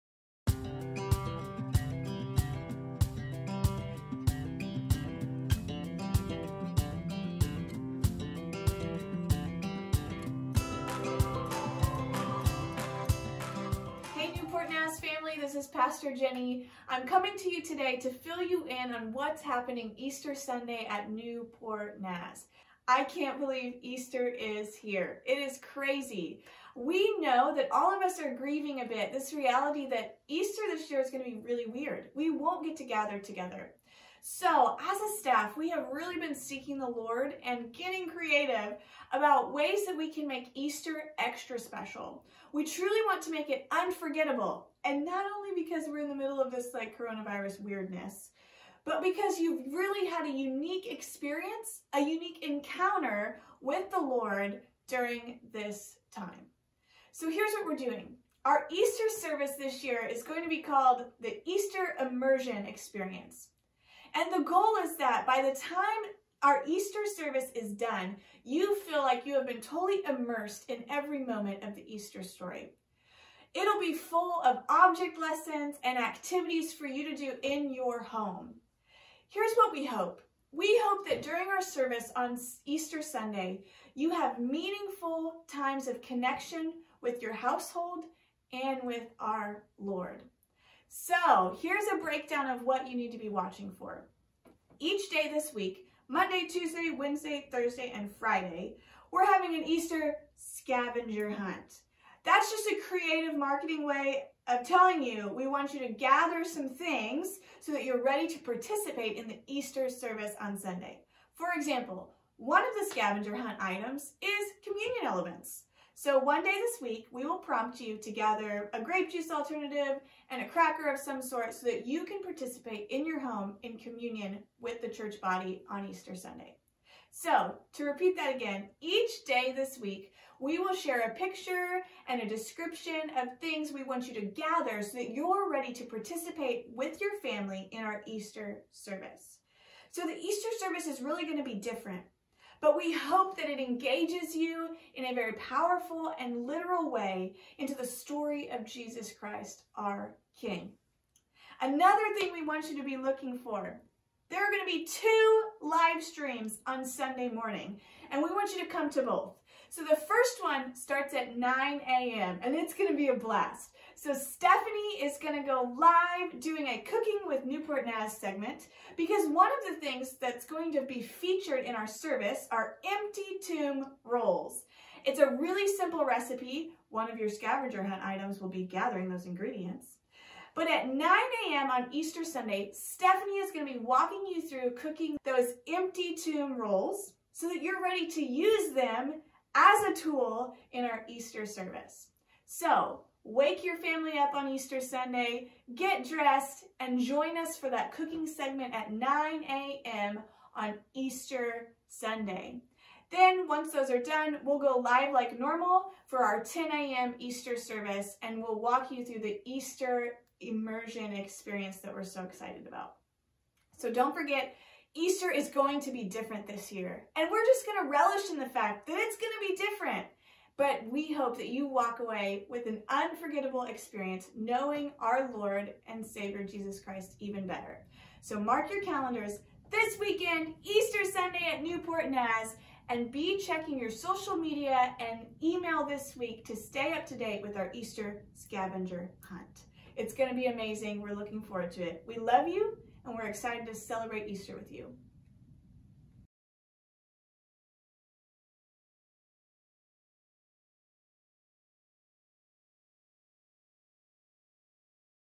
Easter 2020 Information
Take 4 minutes to watch this video from Pastor Jeni explaining Easter 2020 at NewportNaz! We are excited to celebrated Resurrection Sunday in such a creative way with you!